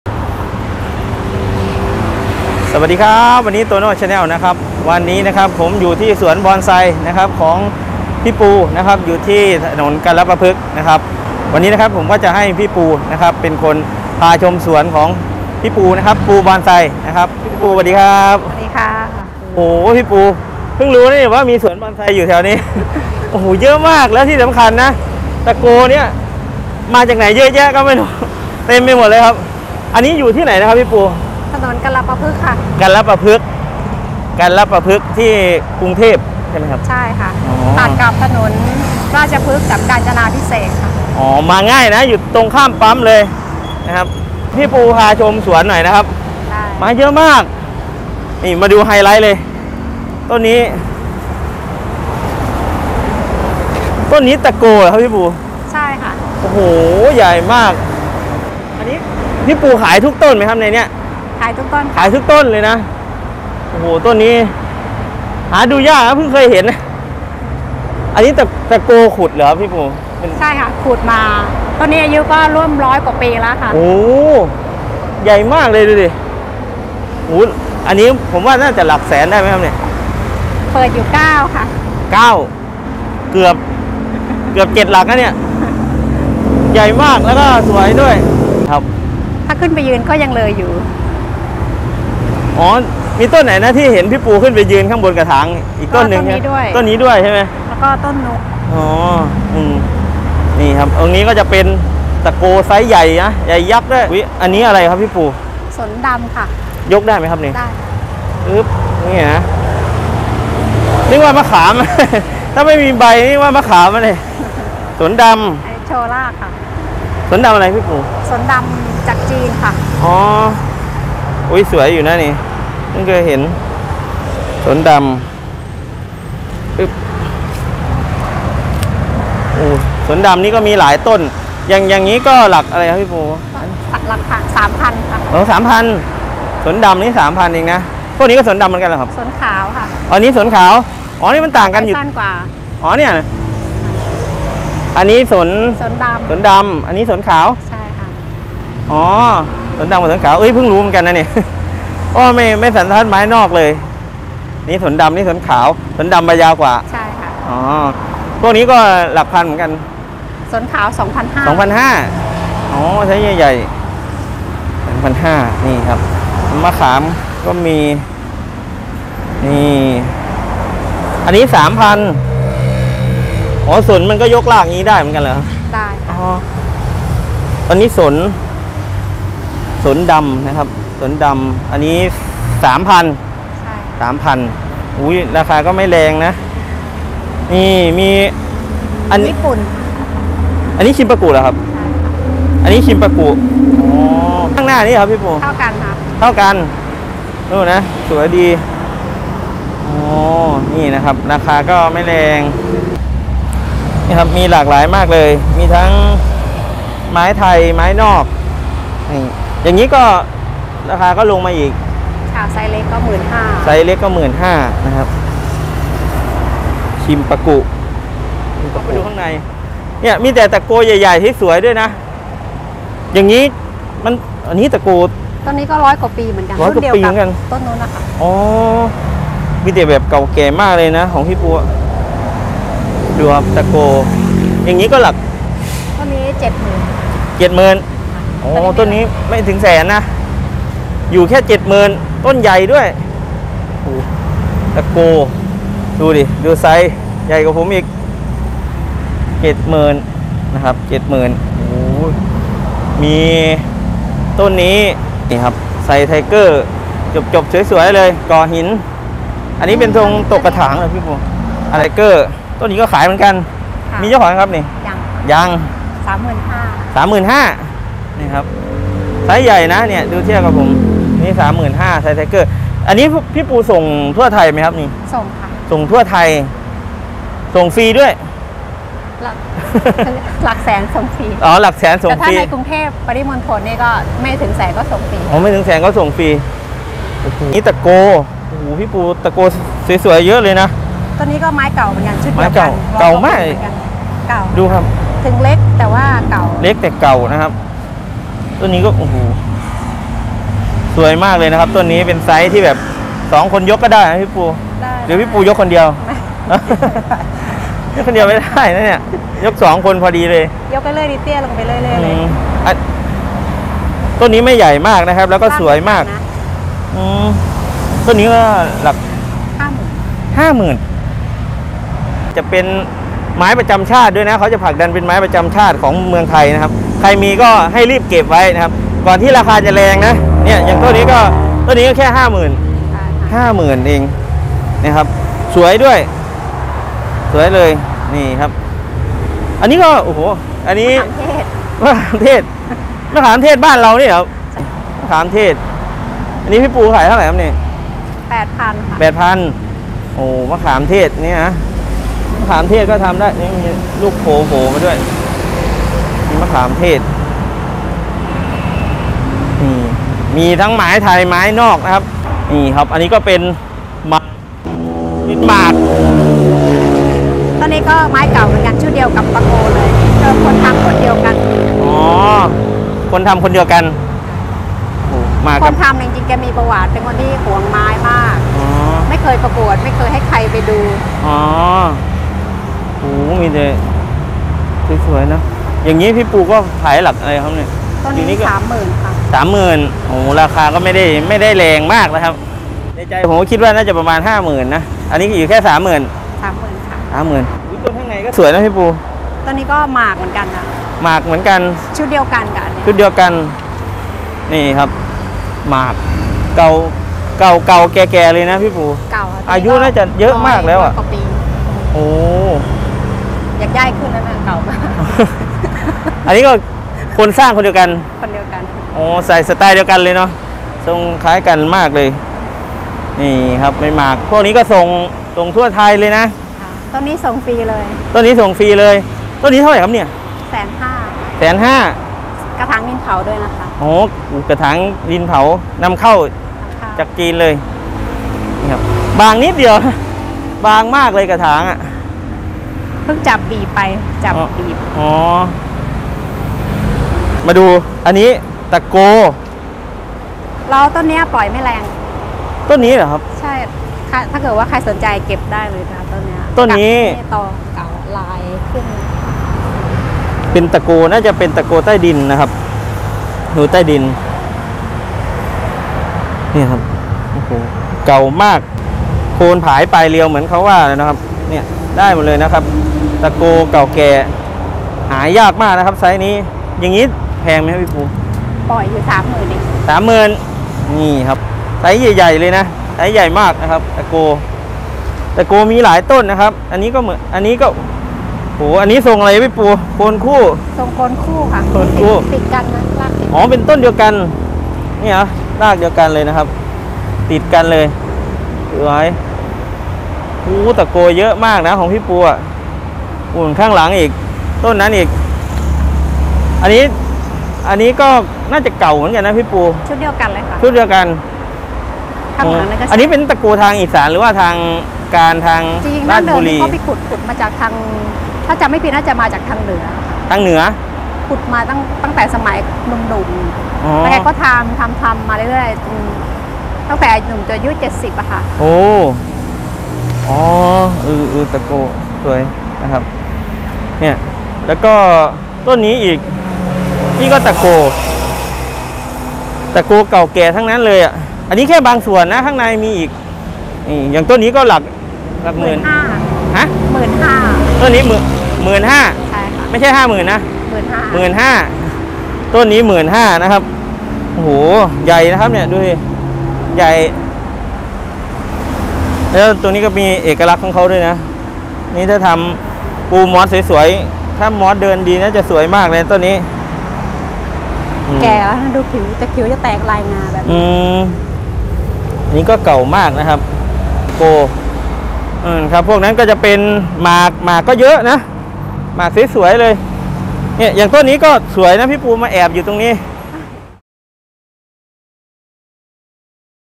สวนบอนไซใจกลางกรุง ปูบอนไซ ตะโก 100 ปี หลักแสน และไม้เก่าแก่เยอะมาก EP1
สวนบอนไซใจกลางกรุง ปูบอนไซ
ตะโก 100 ปี หลักแสน และไม้เก่าแก่เยอะมาก EP1
ติดต่อสวนปูบอนไซ
เพจวิว ปูบอนไซ poohbonsai